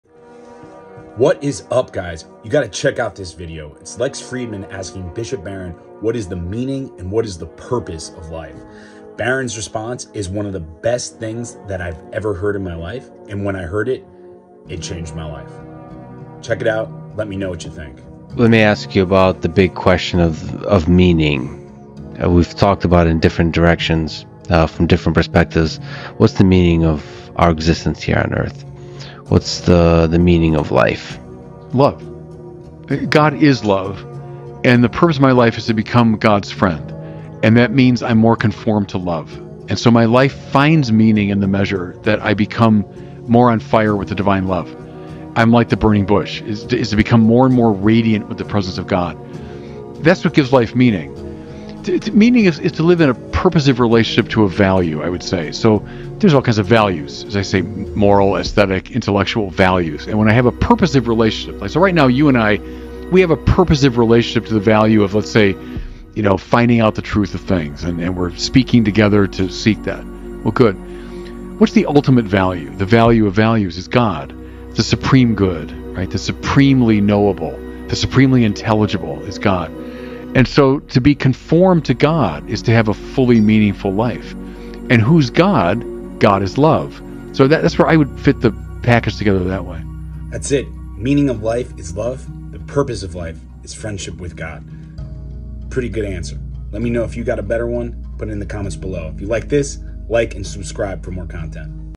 What is the Meaning and Purpose of Life?
Lex Fridman asks Bishop Barron “what is the meaning and purpose of life”. I react to Bishop Barron’s amazing response.